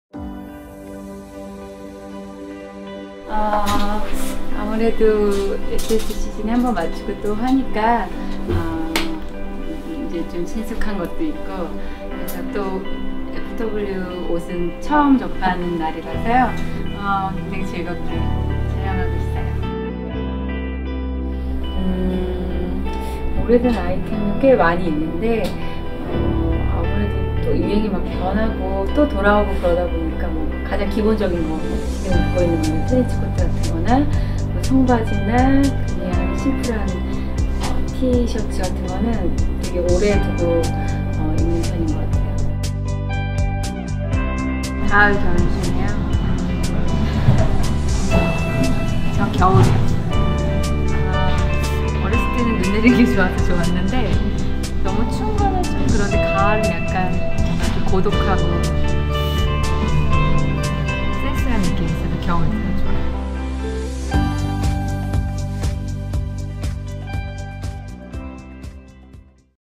한지민(Hanjimin) 아나카프리 F/W 화보촬영장 인터뷰
720HD